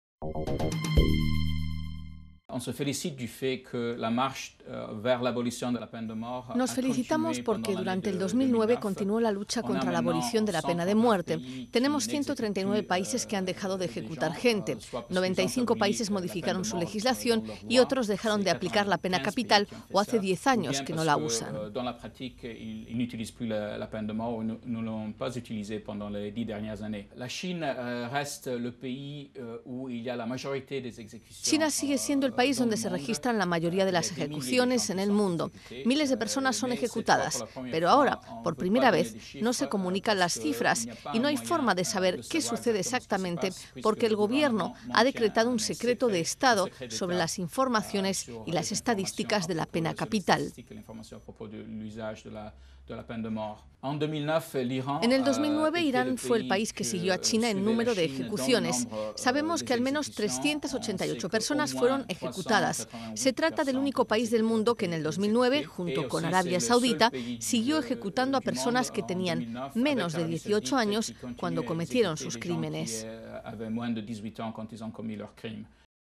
Amnistía señala a China por aplicación de la pena de muerte
Amnistía Internacional instó a China a decir públicamente cuántas ejecuciones lleva a cabo cada año, con ocasión de la publicación este martes de su informe anual sobre la pena de muerte en el mundo.